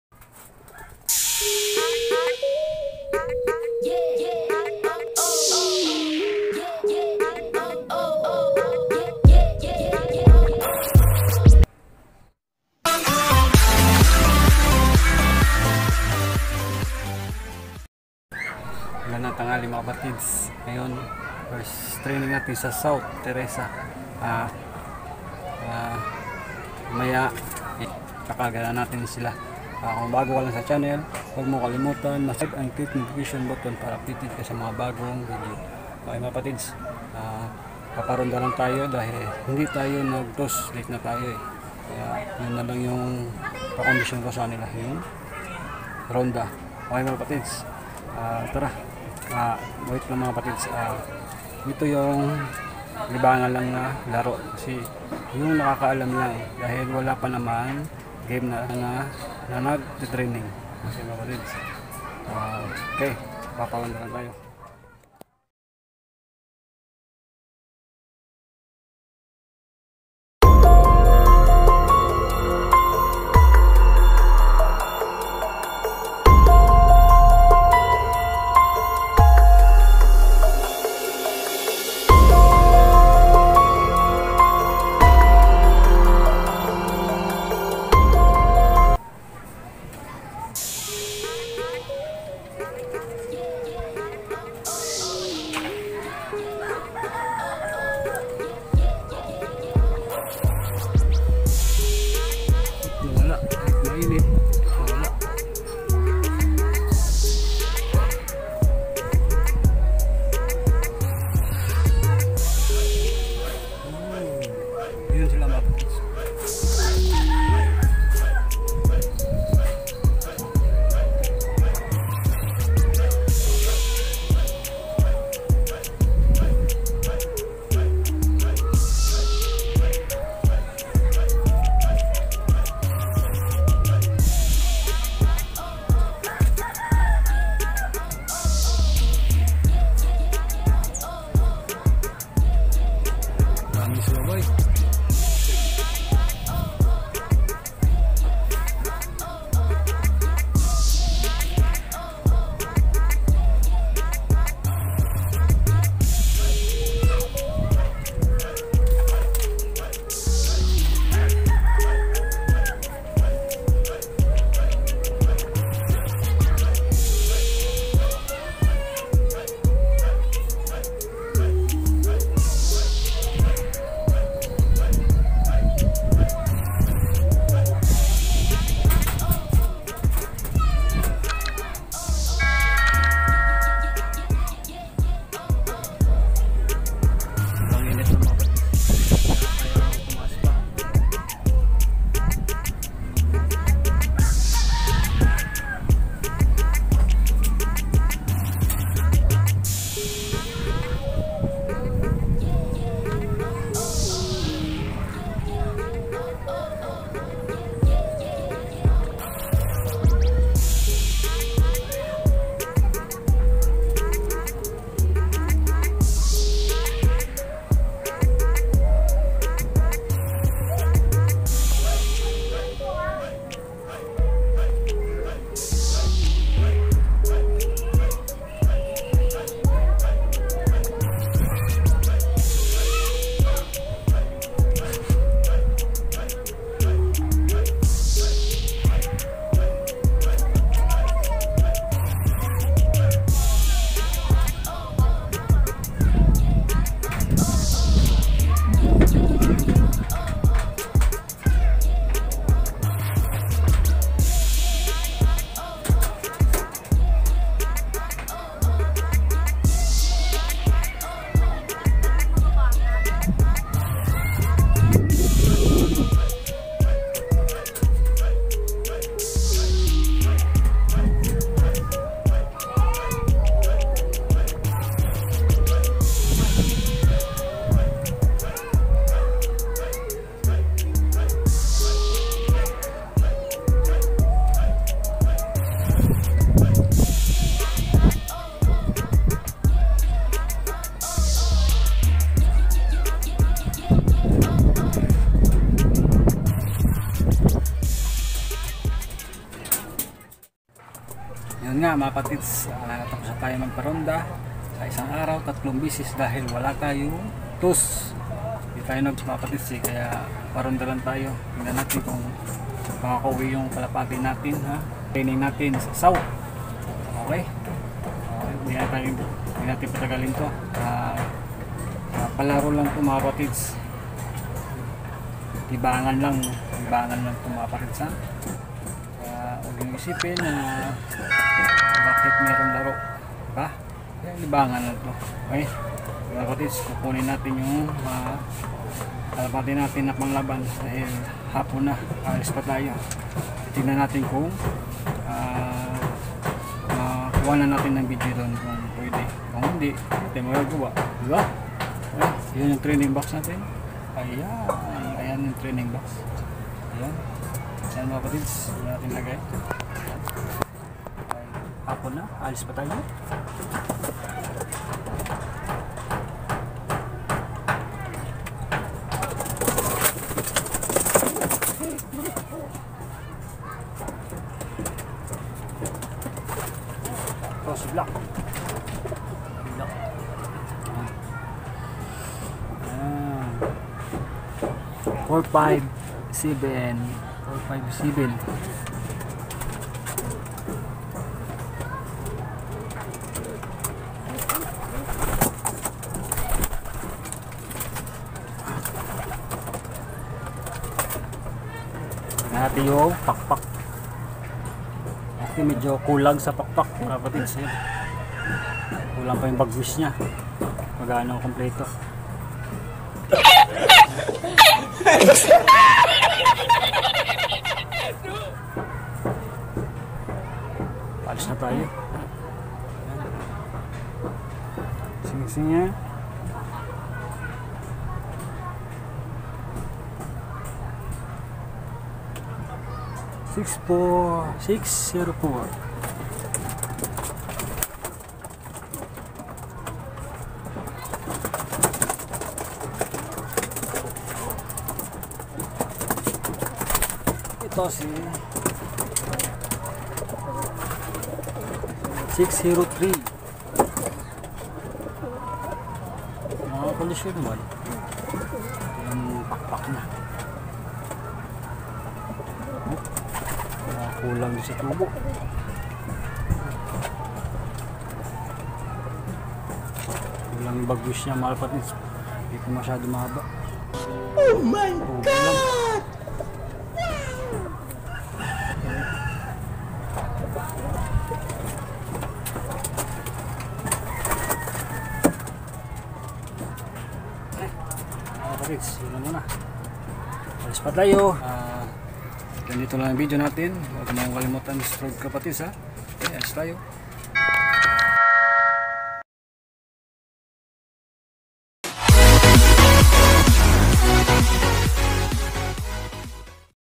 first training south biglaan kayaning kaya kahit walang toss makauwi kaya
helo manga kalapatids paronda lang tayo sa isang araw tatlong bases kayaning kaya ng kalapati natin kahit walang toss abanga nalang natin manga kalapatids god bless all support